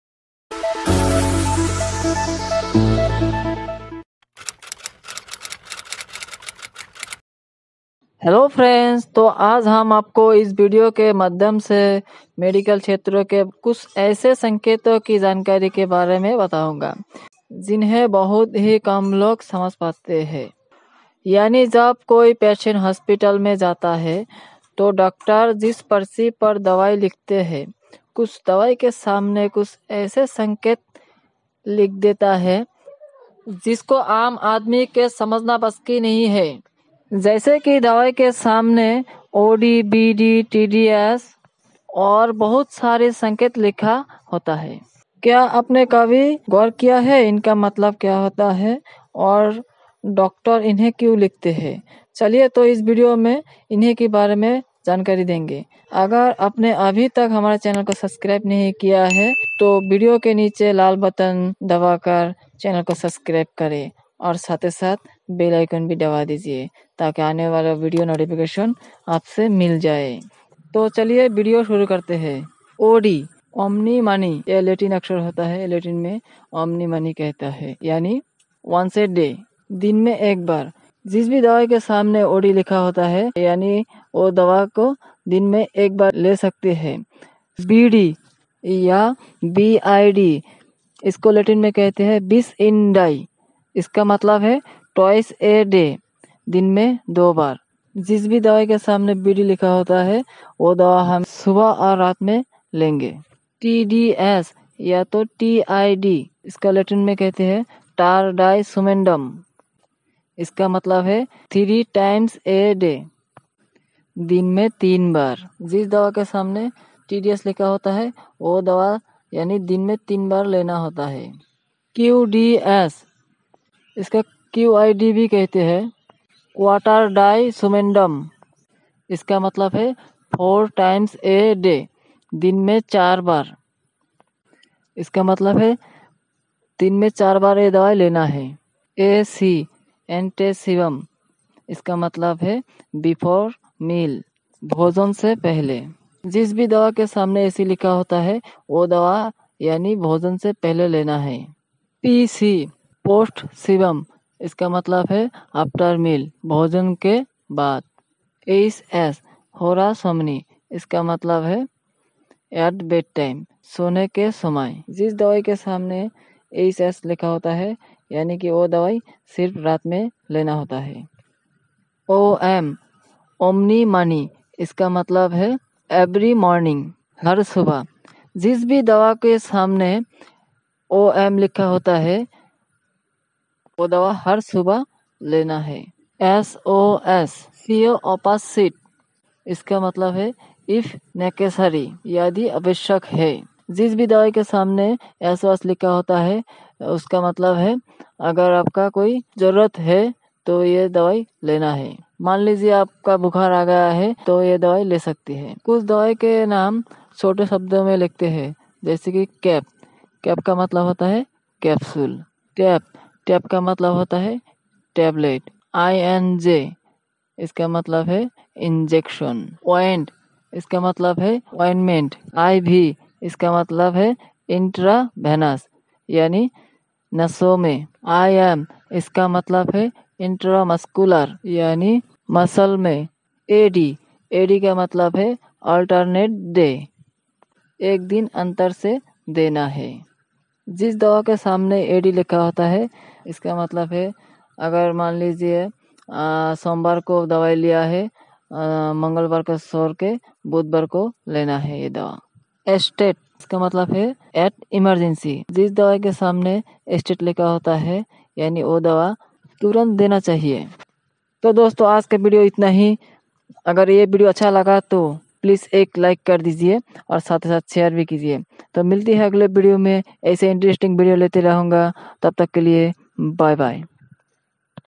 डॉक्टर दवा लिखते समय OD , BD , TDS क्यो लिखते है ।। जानिए क्या है इसका मतलब Hindi में|SKmedicine
Namaskar dosto SK medicine channel mein apko swagat hai
डॉक्टर दवा लिखते समय OD , BD , TDS , QDS,QID,AC,PC,OM,क्यो लिखते है ।। जानिए क्या है इसका मतलब Hindi में od bd tds hs in medical prescription

sorliv syrup use and benefit review in Hindi

skmedicine how to read doctor prescription#